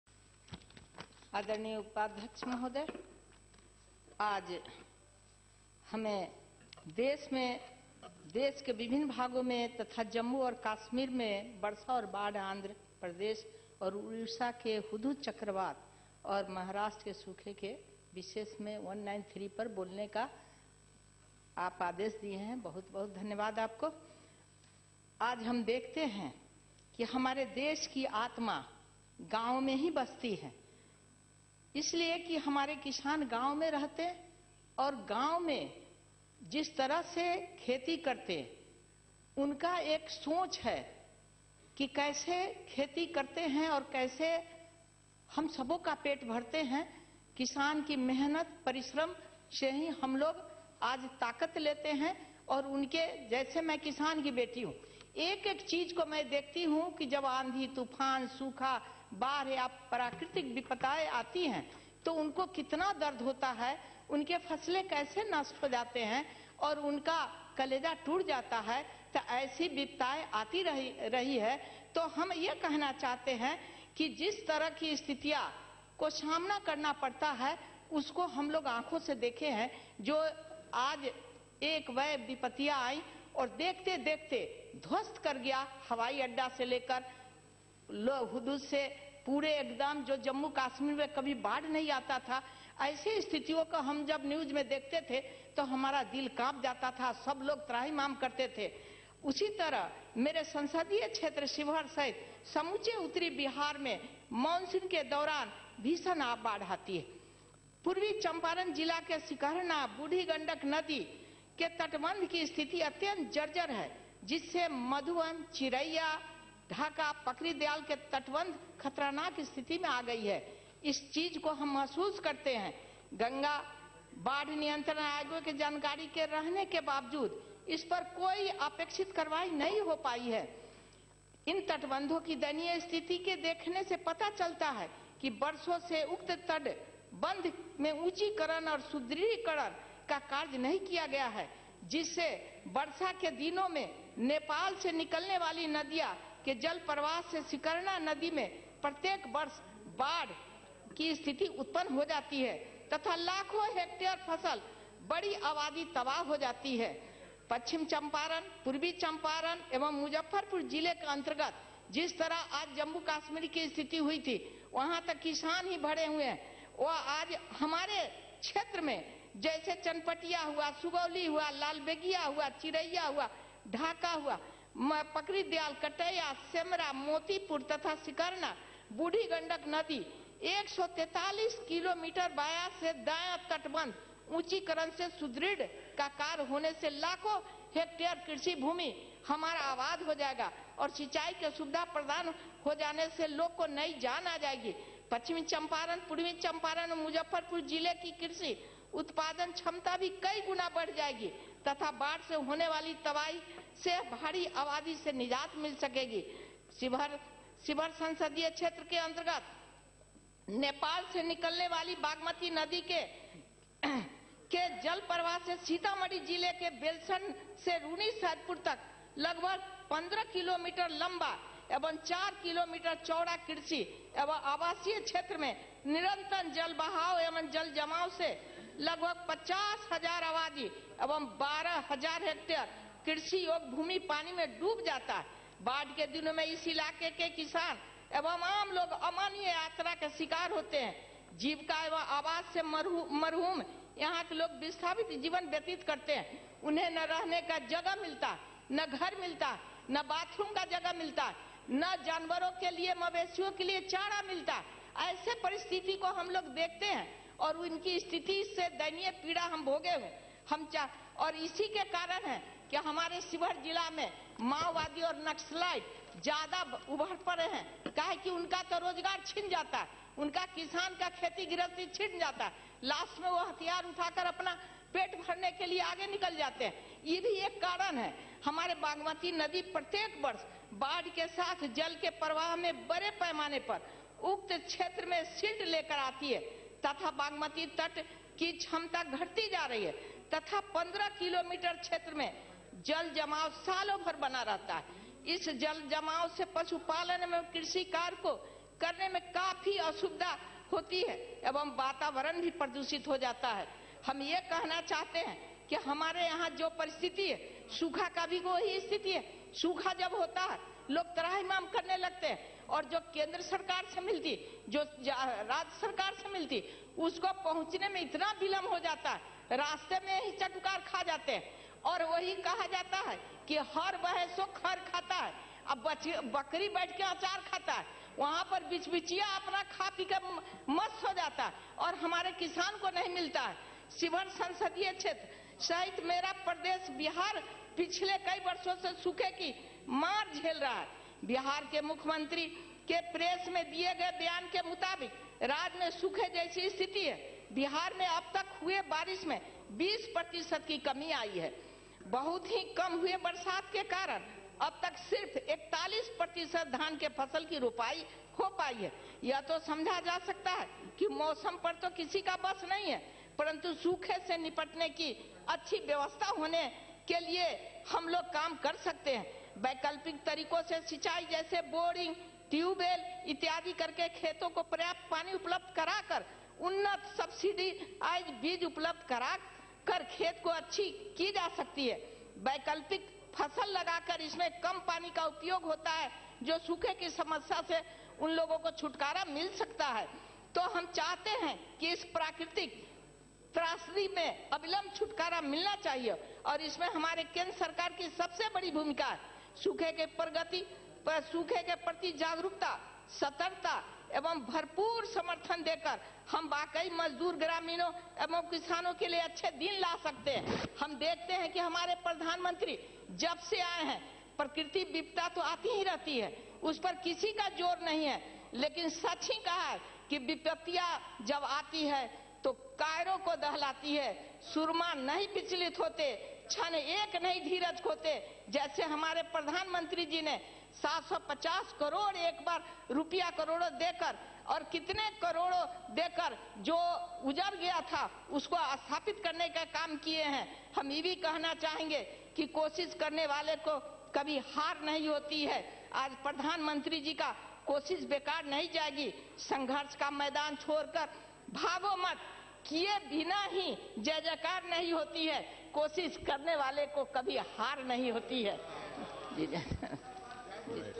Smt Rama Devi on the recent natural calamities faced by different part of the country
5th December 2014: Discussion Under Rule 193 on the recent natural calamities faced by different part of the country: Smt. Rama Devi